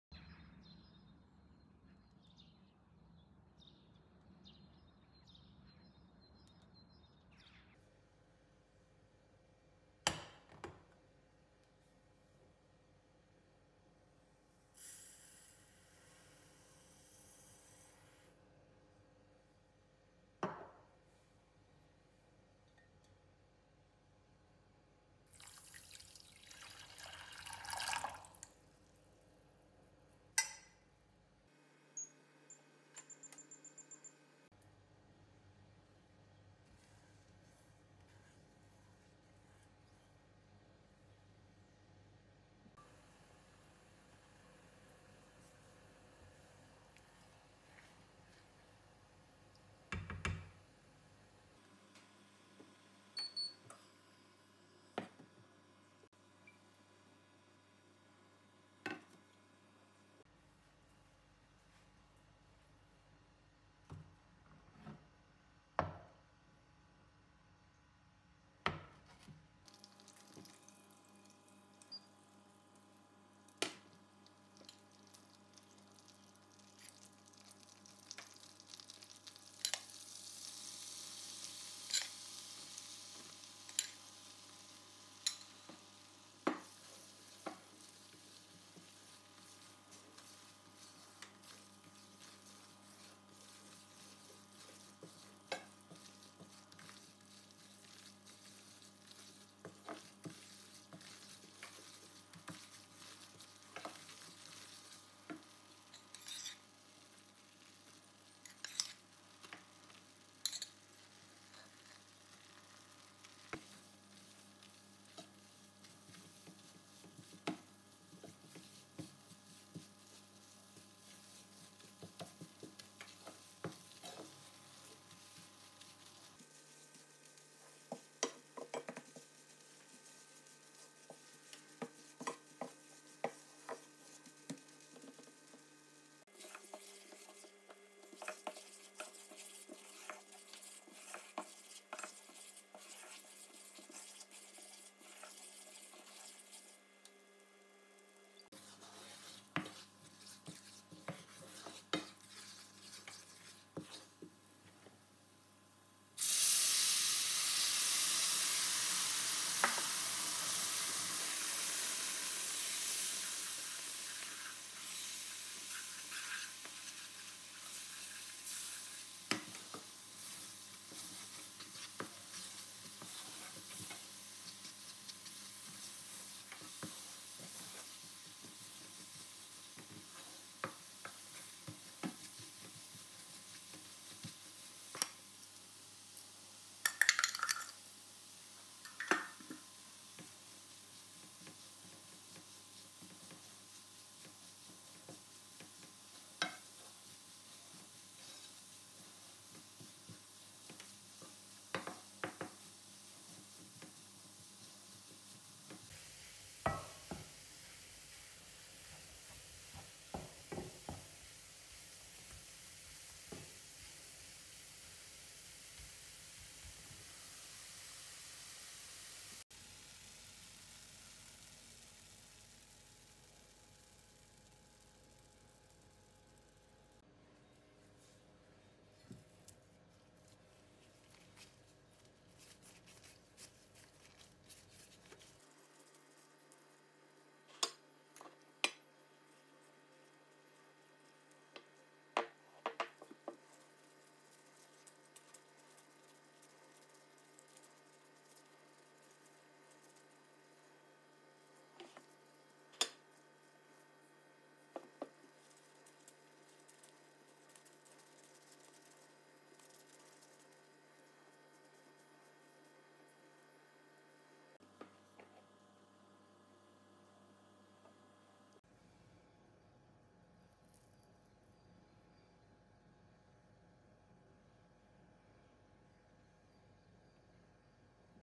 Hallva më e SHIJSHME që do e Provoni 😍 Si na e bënte NËNA dikur 💖
Përbërësit :
120 gr. sheqer
250 ml. uj
100 gr. gjalp
100 gr. miell
3 lugë gj. arra të bluara
i lyejm me kokos
Ju bëfshin mirë në qoftë se i provoni 🫶